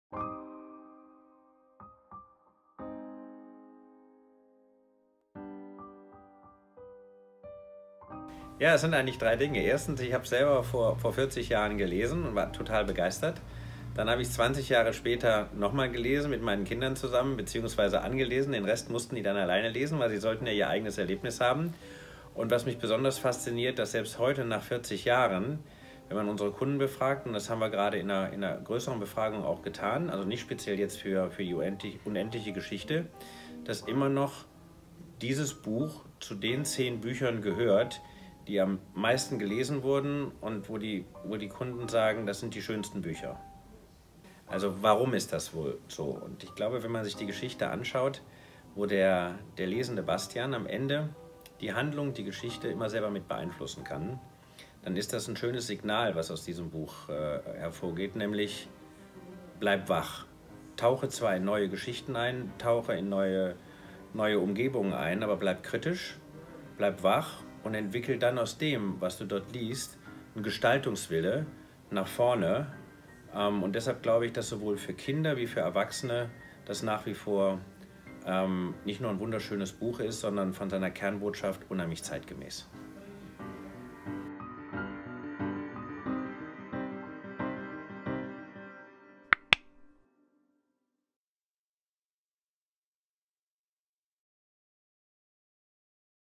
Michael Busch (Thalia) über „Die unendliche Geschichte“
Erinnerst du dich noch an deine erste Reise nach Phantásien? Zum 40-jährigen Jubiläum von „Die unendliche Geschichte“ haben wir über 20 Persönlichkeiten – Prominente, Fans und Wegbegleiter Michael Endes – nach ihren Geschichten gefragt. Entstanden sind ganz persönliche, witzige und sehr berührende Videos, die alle die Liebe zur Phantasie einen.

Ihr habt auch besondere Erinnerungen an den Jugendbuchklassiker? oder unter botschafterphantasiens.